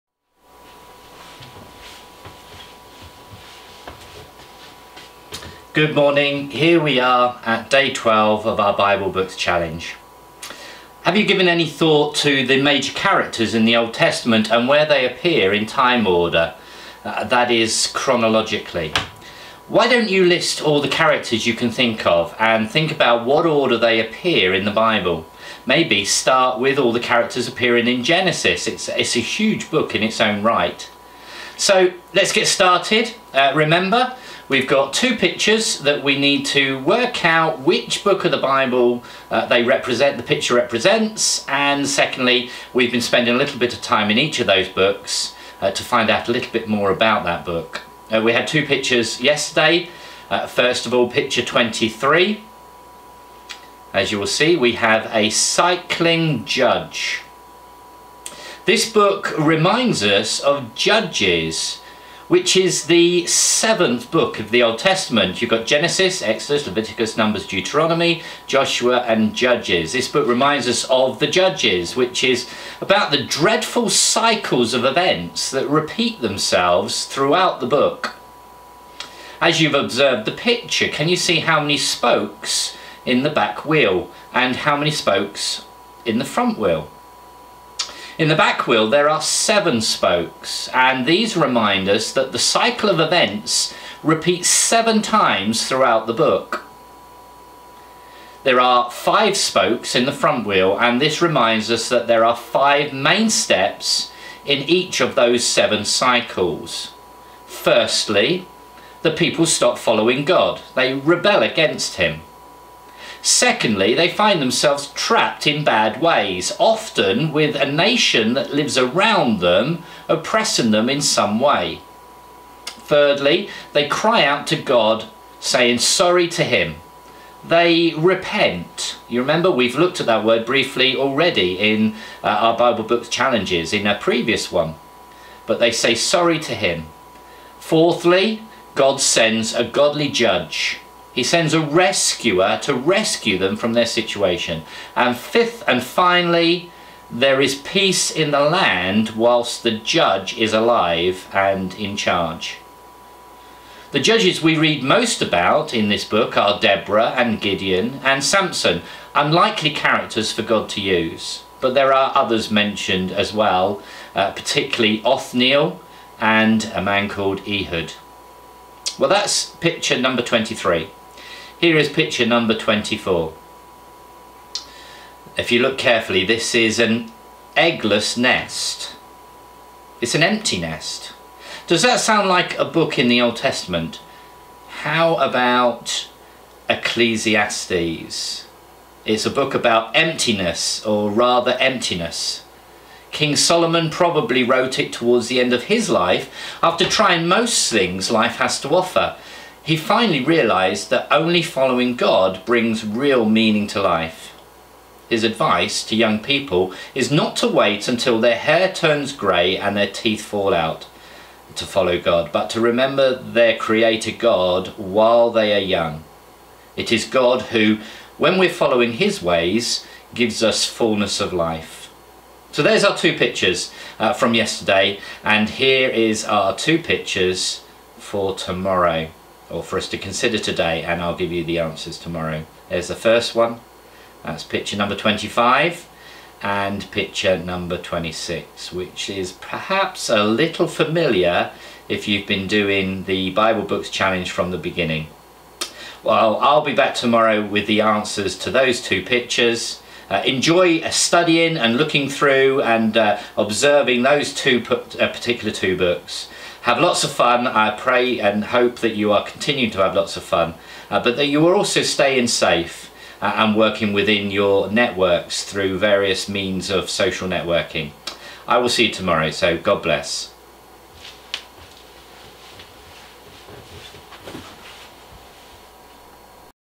BBC Day 12
Bible Books Challenge is a fun way of learning the different books of the Bible. In the same style as 'Catchphrase' or the board game 'Dingbats', you need to work out, from the picture, which book of the Old Testament is being represented.